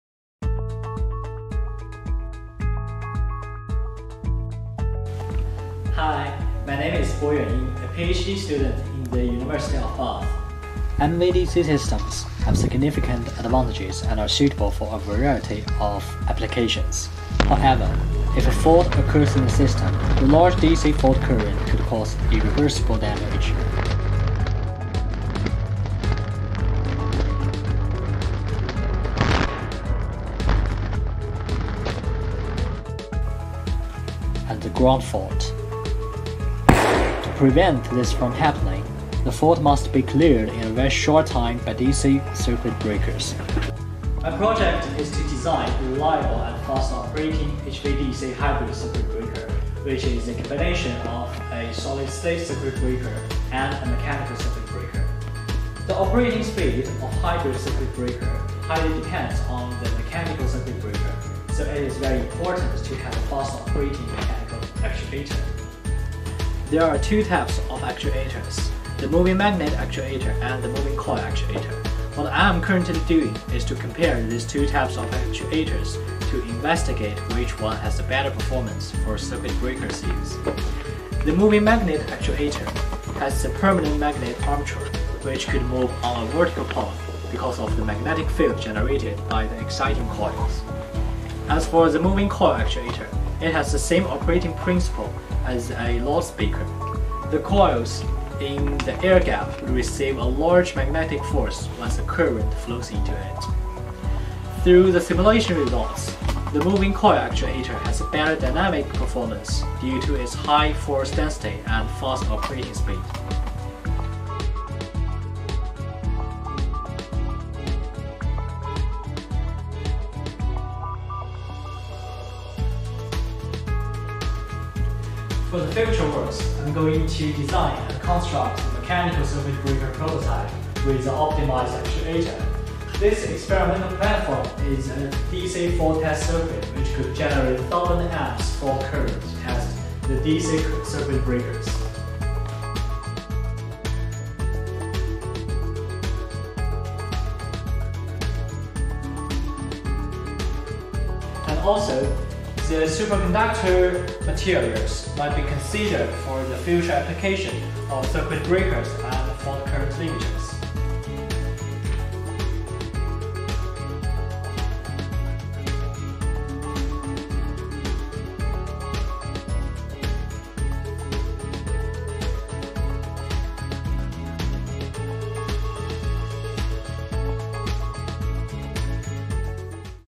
The development of actuators for mechanical circuit breakers
Paper title:
A comparison between moving magnet and moving coil actuators for vacuum interrupters.

Paper abstract:
Medium voltage direct current (MVDC) systems have been proposed for a variety of applications including offshore wind farm collector system and transport electrification. An efficient and reliable hybrid DC circuit breaker is critical for MVDC system protection. The operating speed of the hybrid DC circuit breaker heavily relies on the speed of the mechanical circuit breaker. Two types of actuators: moving magnet actuator and moving coil actuator using electromagnetic interaction are compared in this paper for the operation of a medium voltage vacuum interrupter. The Finite Element software package COMSOL is used to simulate the static and dynamic performance of each actuator topology. The paper focuses on comparing the advantages and disadvantages of these two actuator topologies particularly for vacuum interrupter application. The moving coil actuator is found to have better dynamic performance and is more suitable for medium voltage vacuum interrupter application.